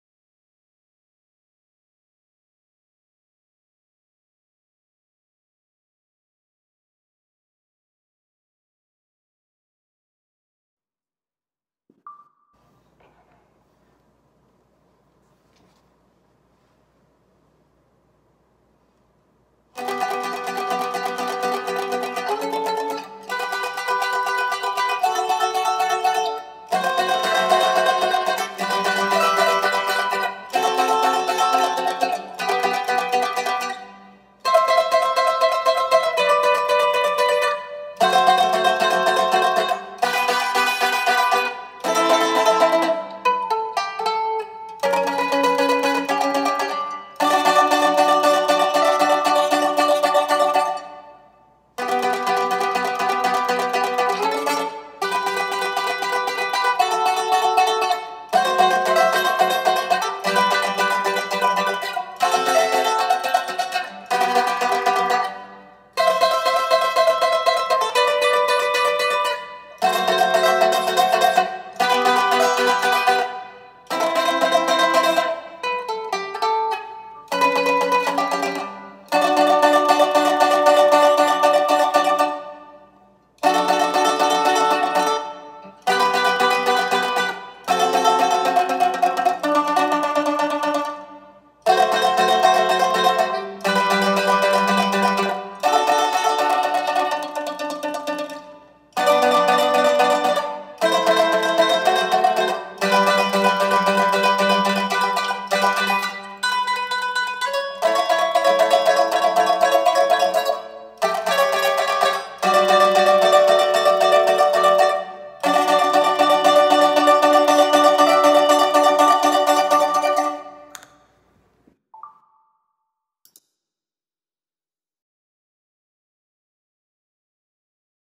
【マンドリンレッスン】初見能力の向上に効果あり！！縦と横の関係を理解して素早く楽譜が弾けるようになろう。
初めての楽譜は、最初に音をとりますが、素早く音をとるためのコツを説明します。ブログに書いています。